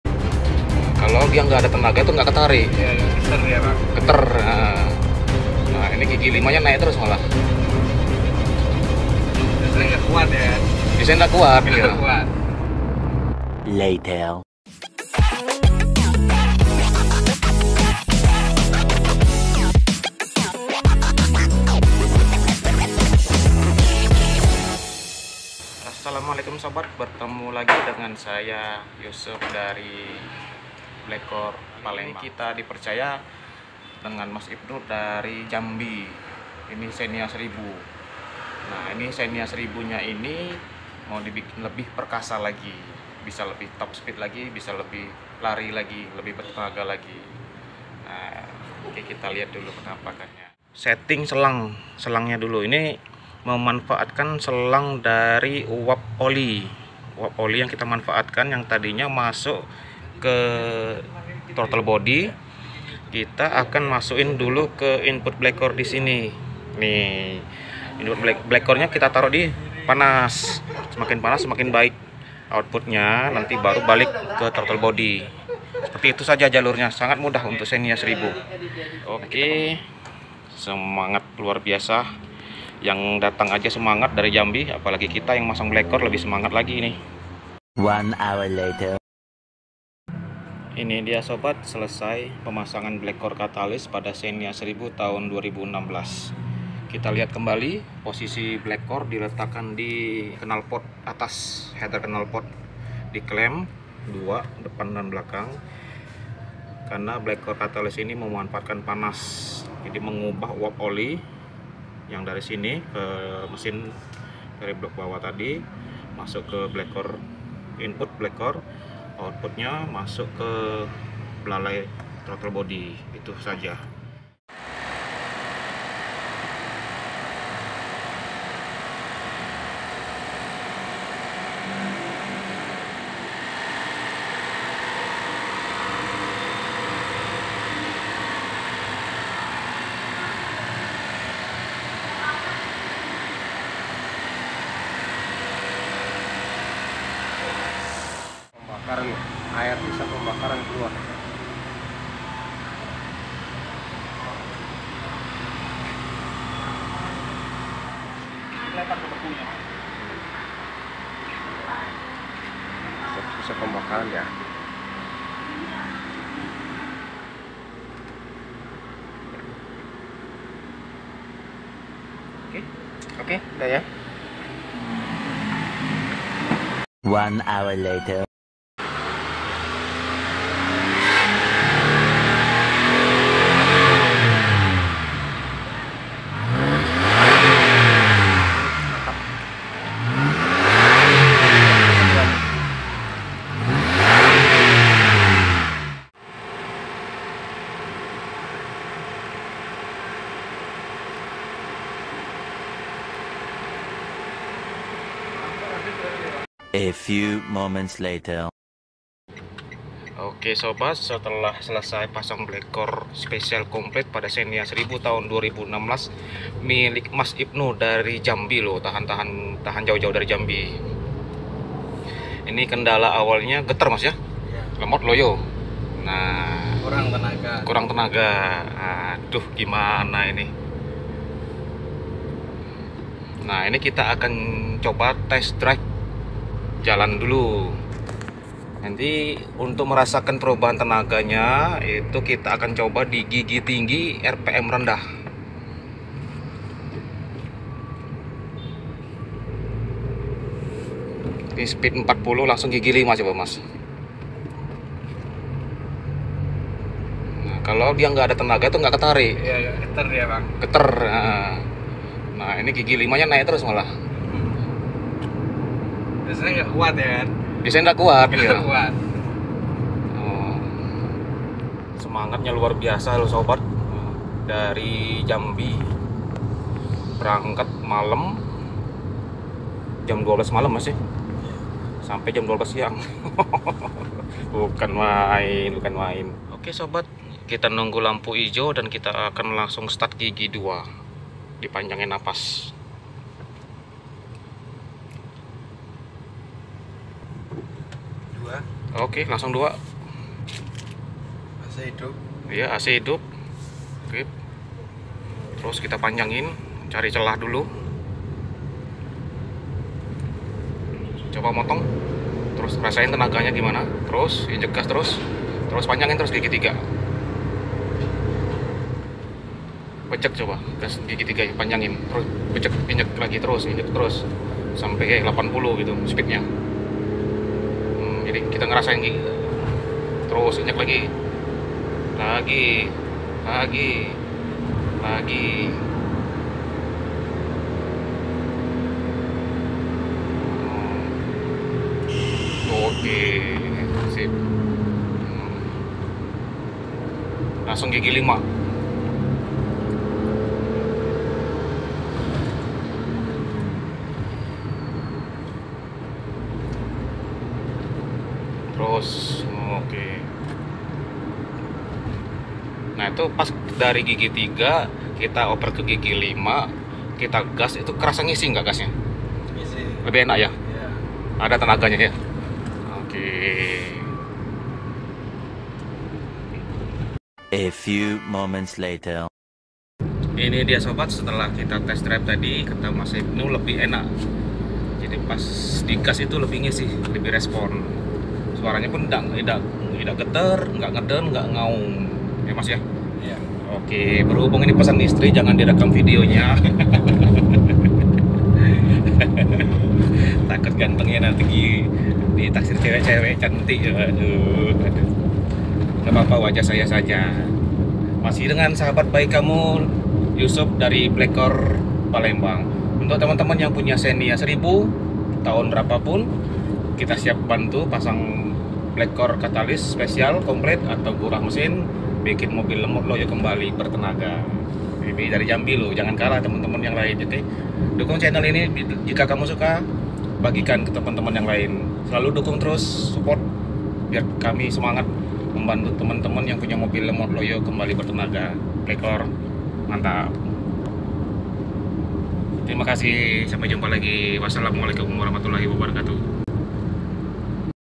XENIA BERTENAGA INOVA SETELAH PASANG BLACKCORE
Xenia 1.0 th 2016 milik mas ibnu dari jambi. Tahan jauh jauh menempuh perjalanan 12 jam ke palembang demi membuktikan Blackcore. Kendala awal mobil geter, lemot, loyo alias kurang bertenaga apalagi saat AC hidup Speed 70 terasa berat banget. sampe segitunya ya...

Setelah pasang Blackcore Spesial Komplit Karakter Xenia berubah menjadi lebih Bertenaga, Tarikan Enteng, Lebih Responsif, Mesin Halus (Tidak Geter), walaupun AC hidup akselerasi mantap.

Torsi dan Tenaga meningkat tanpa perlu bonglar mesin. mantap Blackcore

INFO KONSULTASI /PEMESANAN
Yusuf - Blackcore Palembang